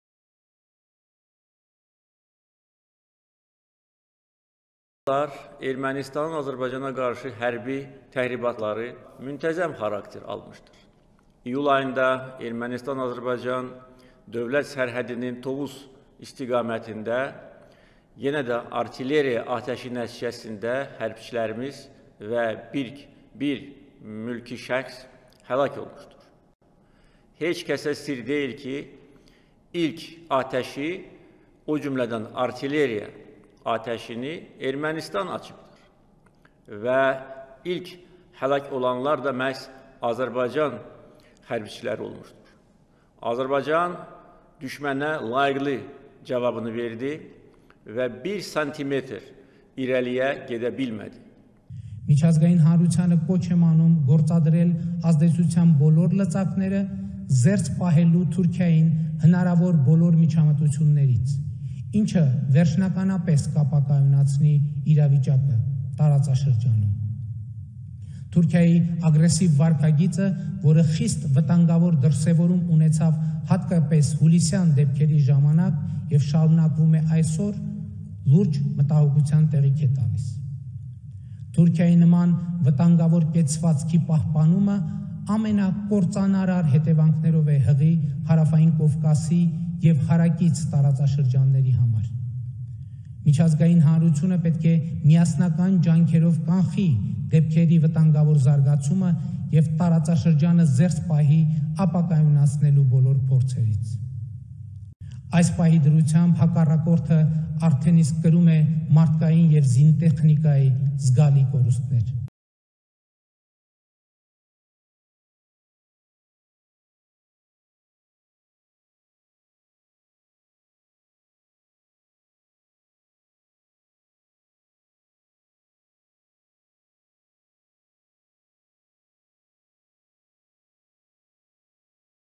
Armenia-Azerbaijan Conflict: Clashes erupt between Armenia and Azerbaijan over disputed region
Azerbaijan’s President Ilham Aliyev said Sunday that Armenia was first to open fire as clashes erupted between the two countries over the disputed Nagorno-Karabakh region.

“Aberbaijan gave a decent response to the enemy and the enemy could not move an inch,” Aliyev said.

Armenia has disputed the claims and said that Azerbaijan carried out an air and artillery attack in the region to which Armenia responded, seizing control of up to seven villages. A claim which Nagorno-Karabakh has denied.

Armenia’s Prime Minister Nikol Pashinyan on Sunday called on the international community to “prevent with united efforts” Turkey’s involvement in the conflict between the two countries, after the latter sharply criticized Armenia.

Pashinyan said Turkey’s behaviour could have “devastating consequences for the South Caucuses and adjacent regions.” September 27th, 2020.